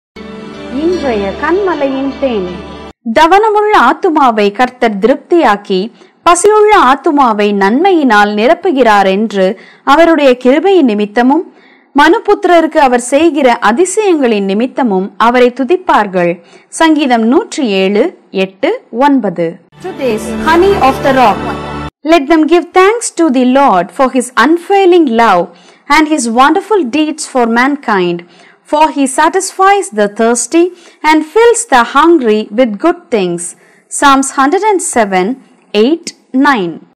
Today's Honey of the Rock | JTK TV | Dec 22, 2020| # Daily Words
Verse 22:
நன்றியறிதல்
தவனமுள்ள ஆத்துமாவைக் கர்த்தர் திருப்தியாக்கி, பசியுள்ள ஆத்துமாவை நன்மையினால் நிரப்புகிறாரென்று, அவருடைய கிருபையினிமித்தமும், மனுபுத்திரருக்கு அவர் செய்கிற அதிசயங்களினிமித்தமும் அவரைத் துதிப்பார்களாக. சங்கீதம் 107:8,9
THANKSGIVING
Let them give thanks to the Lord for his unfailing love  and his wonderful deeds for mankind,
for he satisfies the thirsty and fills the hungry with good things. Psalms 107:8,9